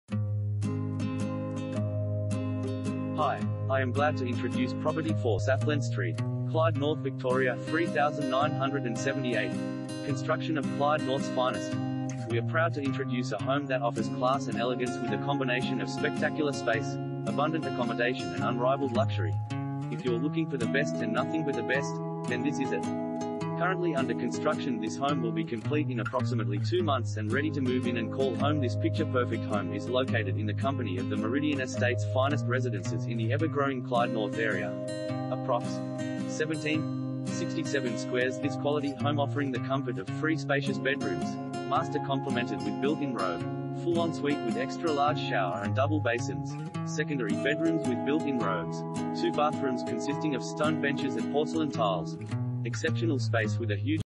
4 Sallen Street, Clyde North VIC 3978 | AuPropertyReport.Com
4 Sallen Street, Clyde North VIC 3978
Property Type: house
Bedrooms: 3
Bathrooms: 3
Car Space: 2
Construction of Clyde Norths Finest!